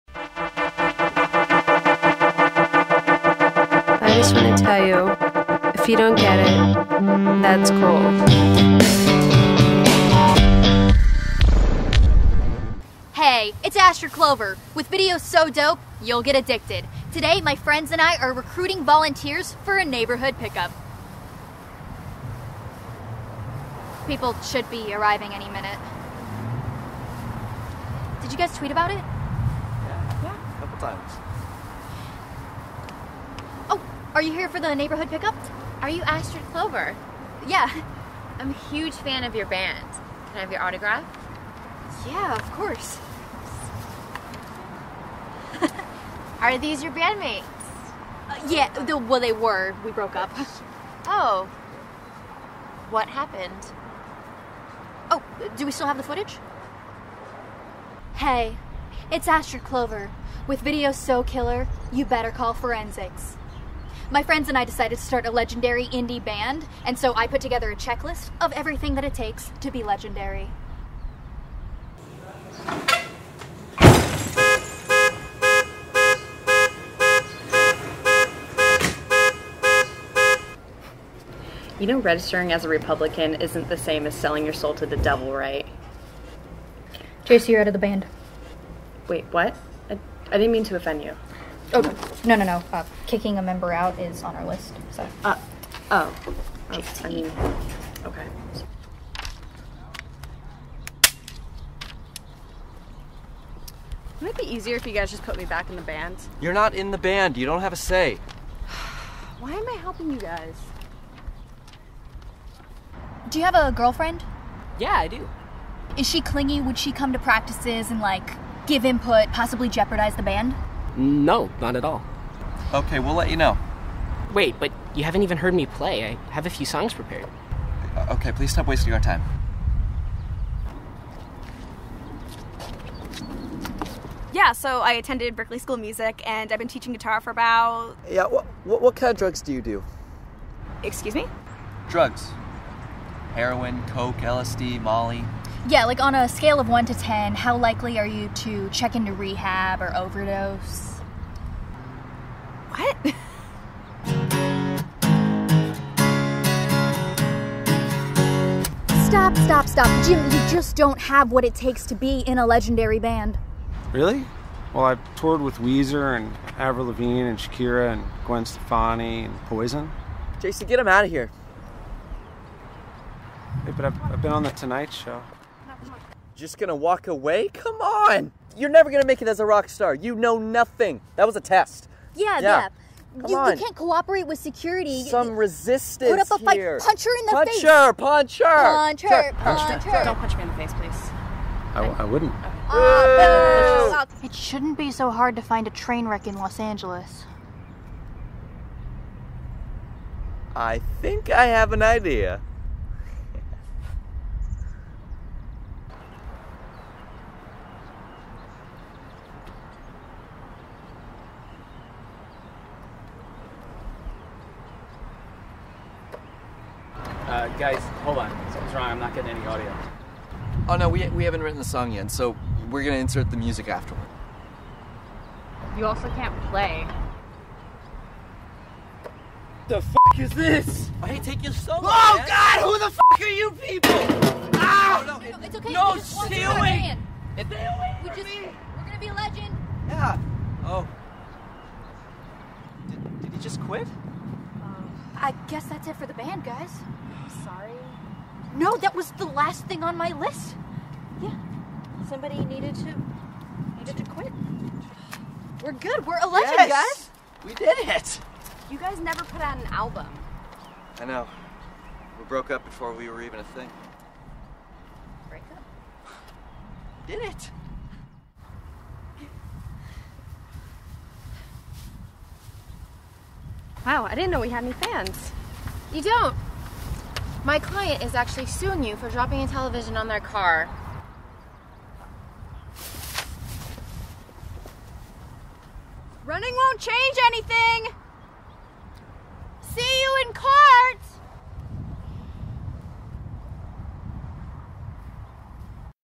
Astrid Clover - Legendary Band
From throwing TV's out windows to selling their souls to the devil, Astrid, Beckett & Jacey follow steps to become a legendary band.